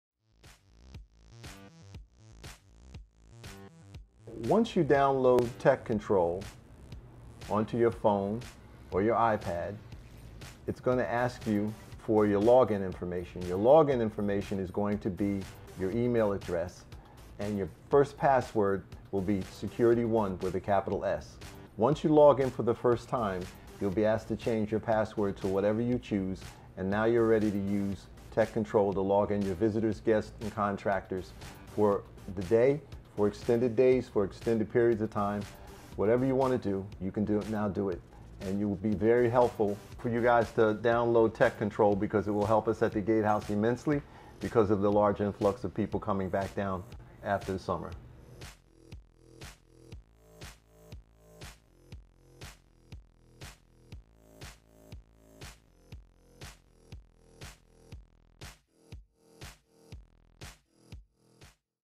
How To: TekControl Access Control Mobile App
Learn how to allow your guests, vendors, family & friends access to the community with the push of a button.